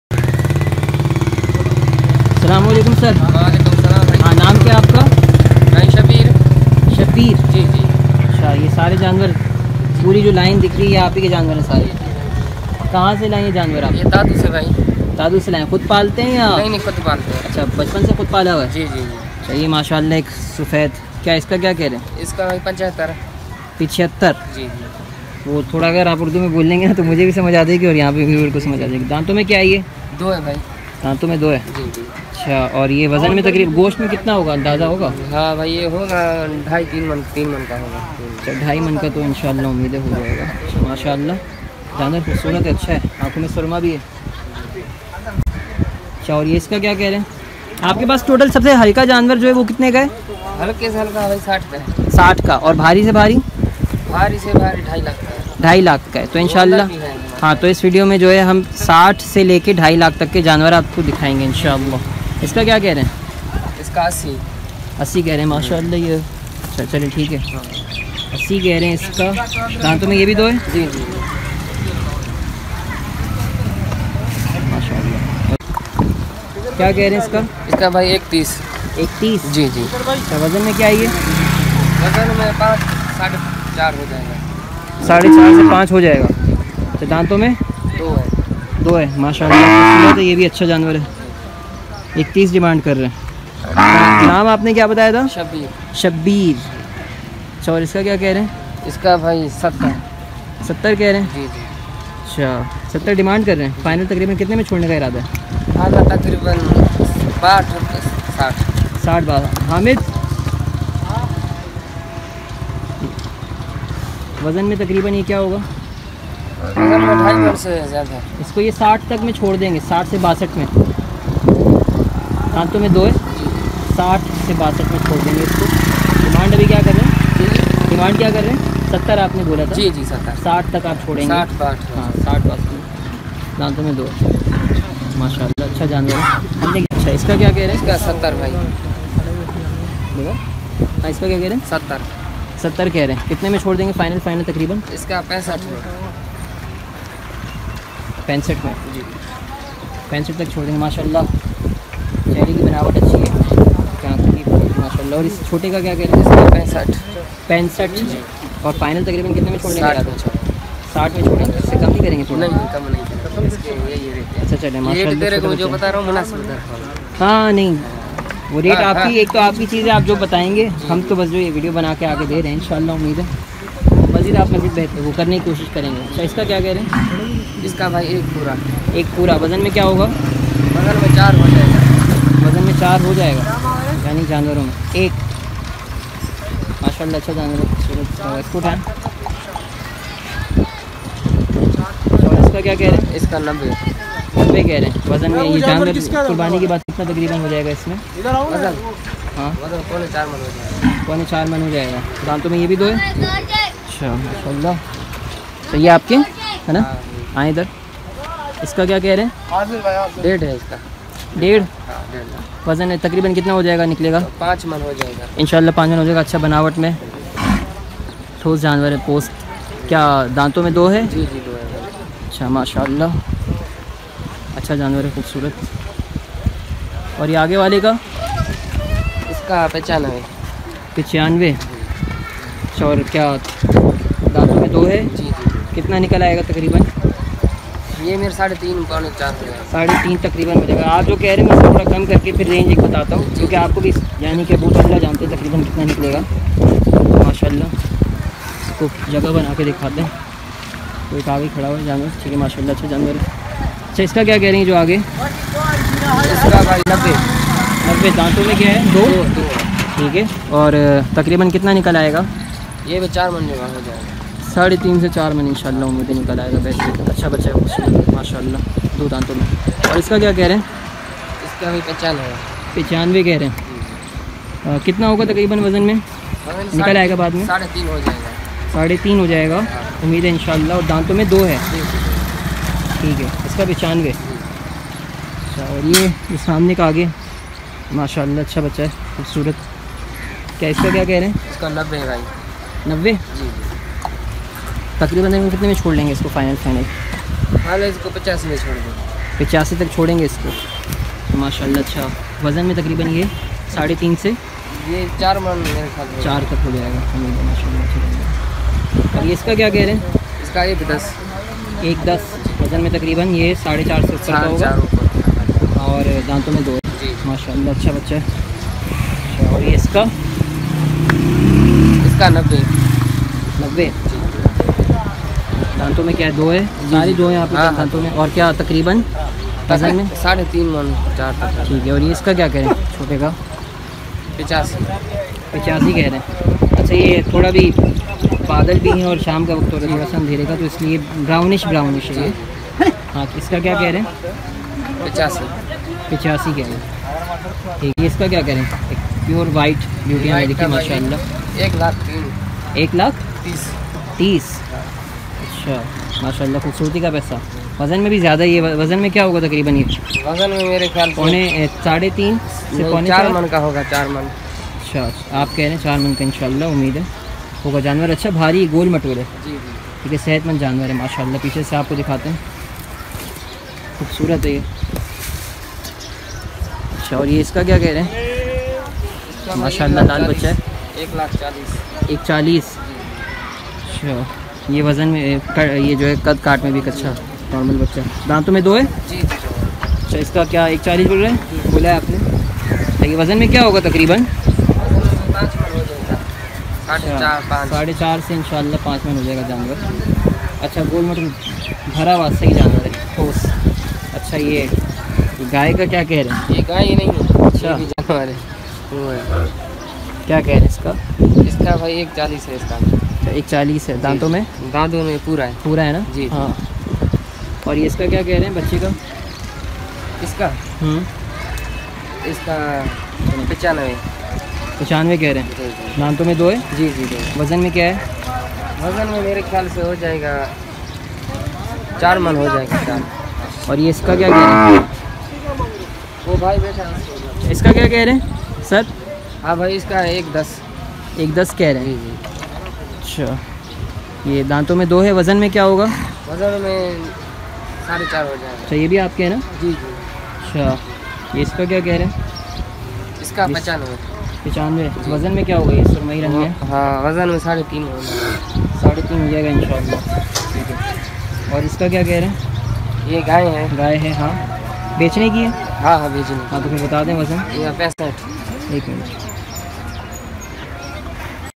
Naval Cow Mandi Karachi 2020 Rates Updates 4th July 2020 | Yousuf Goth Cow Mandi 2020
Qurbani Bull Rates from 60000 to 1 Lac
Naval / Yousuf Goth Mandi Karachi Latest Updates